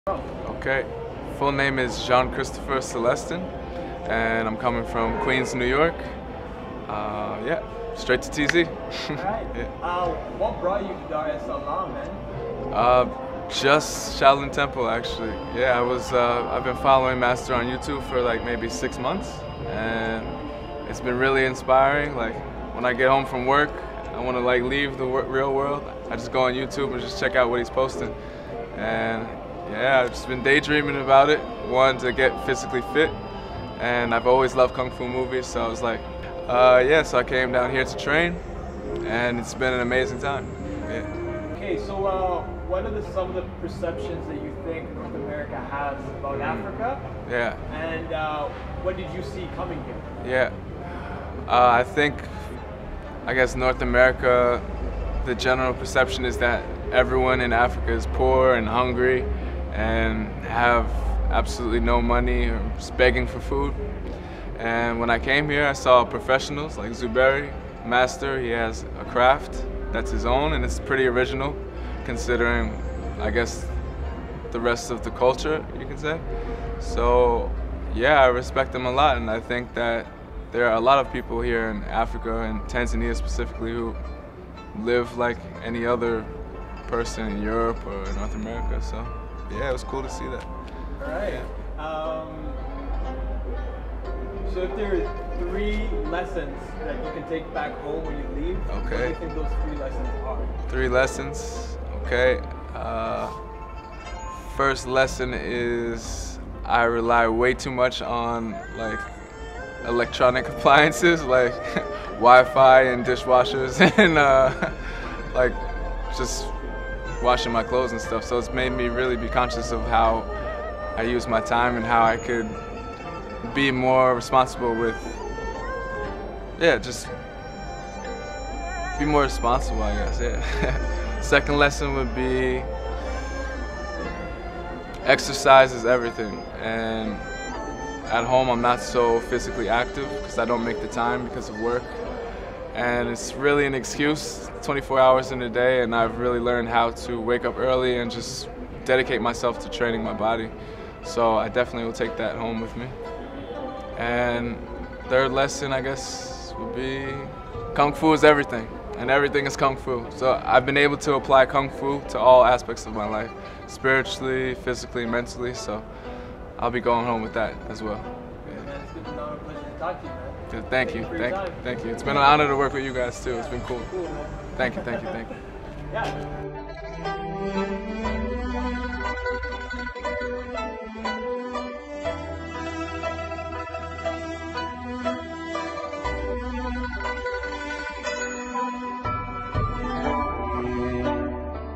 JC'S LIFETIME EXPERIENCE WITH SHAOLIN TEMPLE TANZANIA.
Tembea uone tembea ujifunze.

J.C. akieleza namna alivyojifunza maisha ya kiafrika {Tanzania}

Maisha ni mikasa matukio ndiyo madarasa.